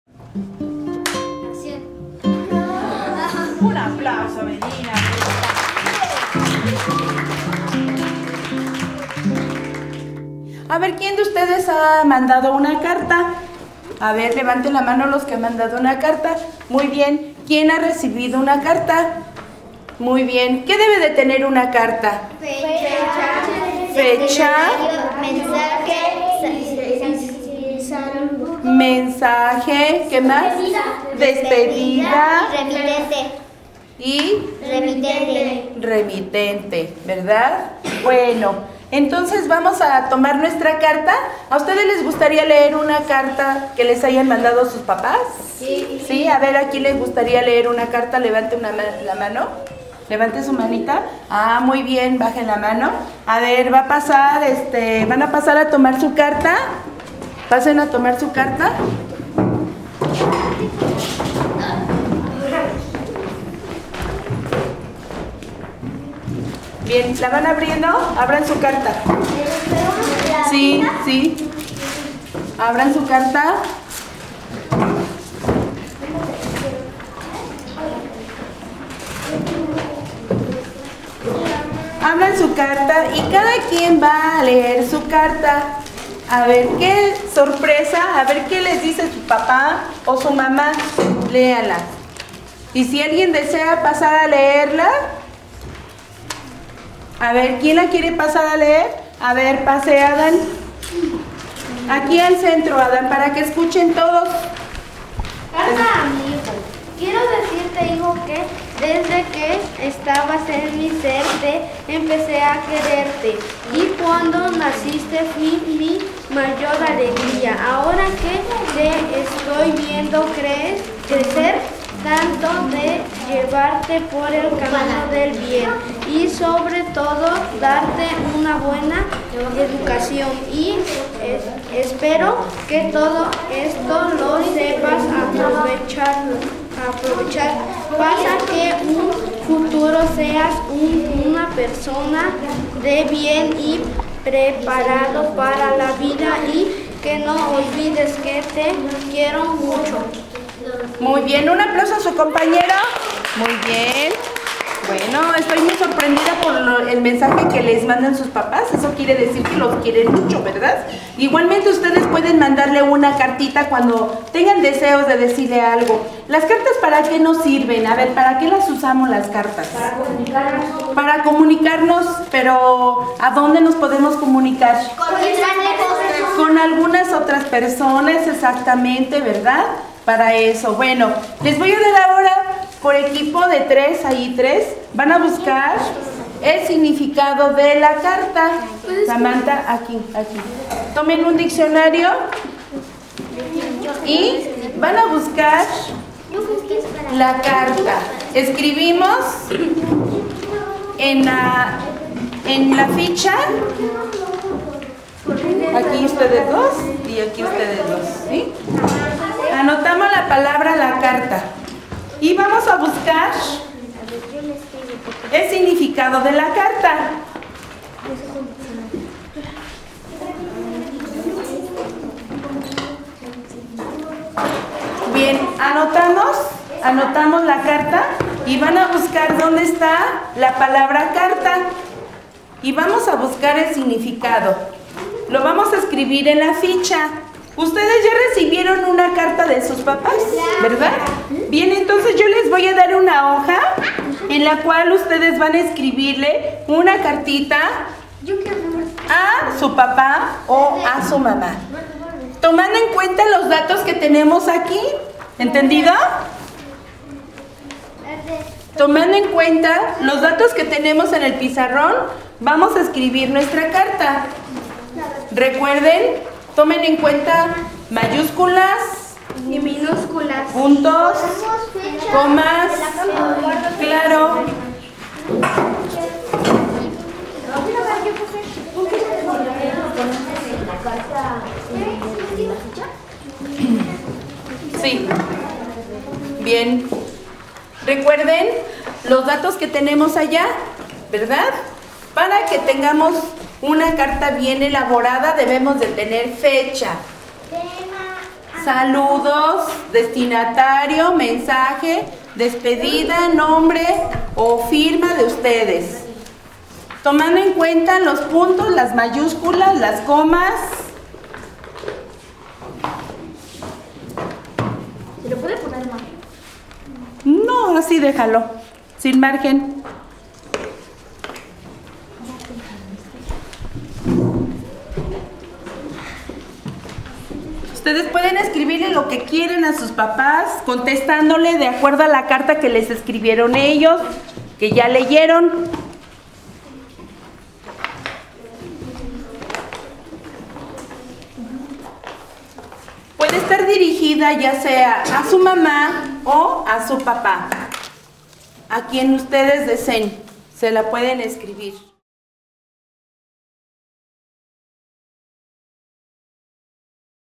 CLIPS : La carta - 2 : Función del tipo de texto
País: México
Ciudad: Ciudad de México
Escuela: Gustavo Diaz Ordaz
Grado: 3ro
Maestro/a: María Concepción Fragoso
Tema: Lengua, Lectura

CLIPS : Banco de registros digitales de prácticas de enseñanza en América Latina
OEI, Universidad Católica de Uruguay / Instituto de Evaluación Educativa